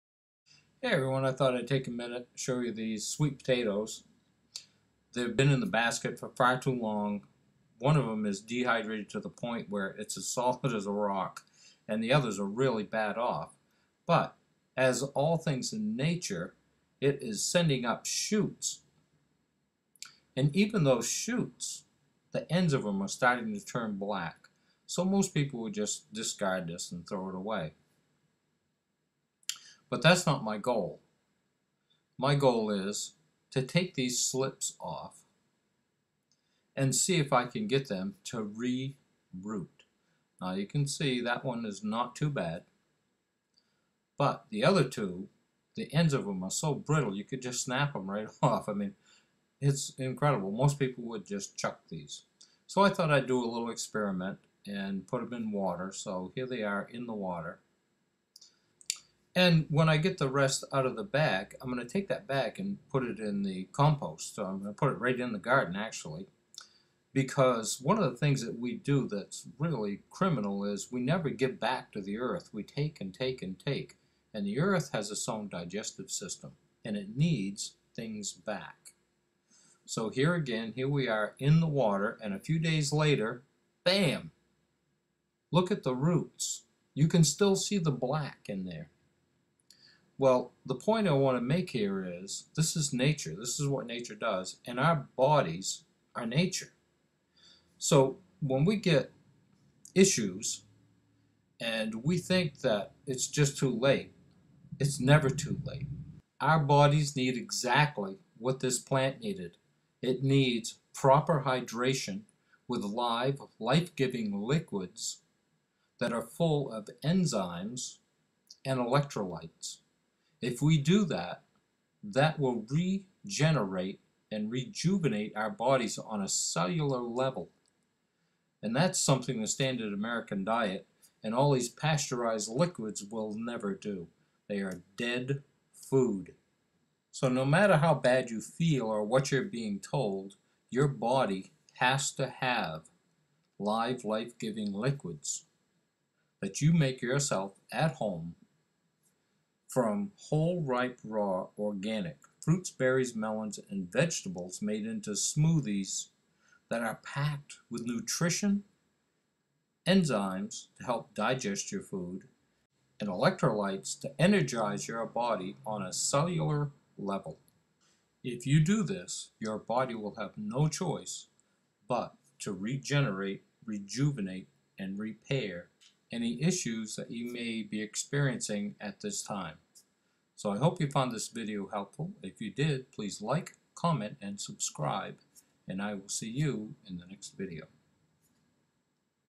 It's Never To Late To Regenerate And Rejuvenate
Live, life giving liquids made from whole ripe raw organic fruits, berries, melons and vegetables made into juices and smoothies buy you at home, are one of the most, if not the most important thing you will ever do to regenerate and rejuvenate your body on a cellular level.